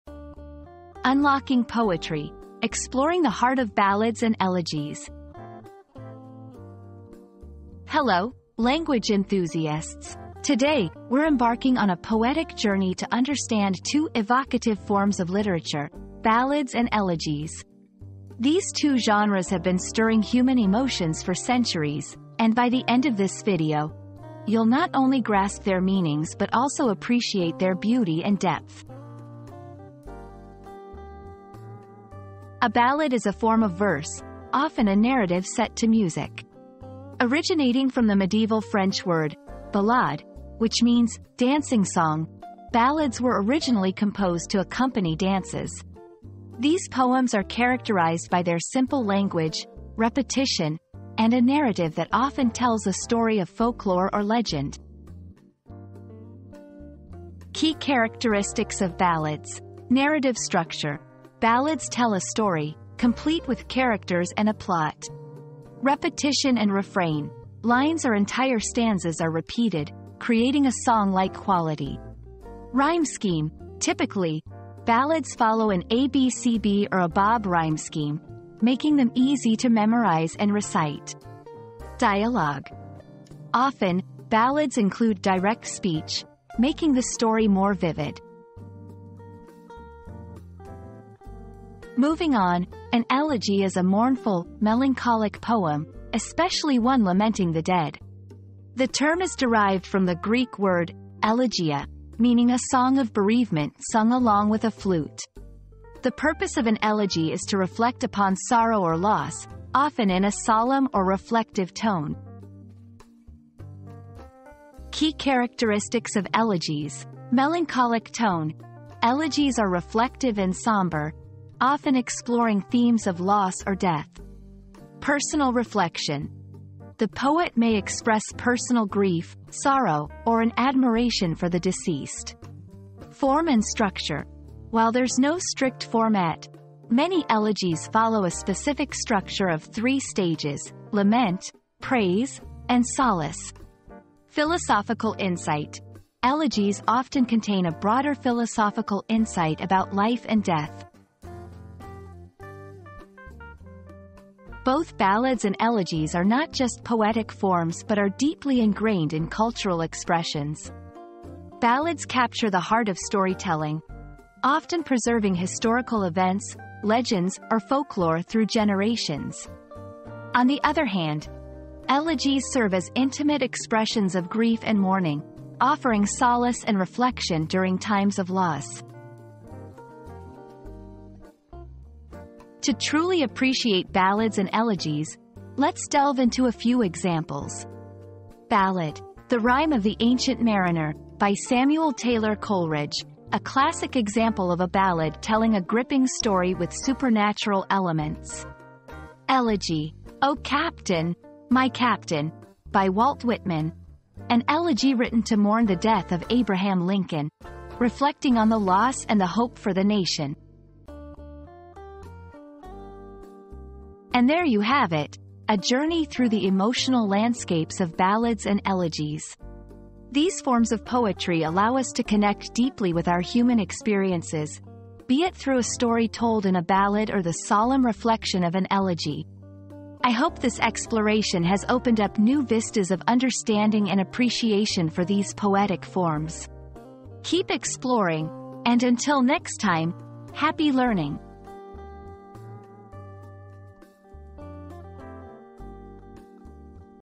Unlocking Poetry: Exploring the Heart of Ballads and Elegies
00:00 • Introduction - Unlocking Poetry: Exploring the Heart of Ballads and Elegies
00:32 • What is a Ballad?
01:31 • What is an Elegy?
02:34 • The Cultural Significance of Ballads and Elegies
03:02 • Examples in Literature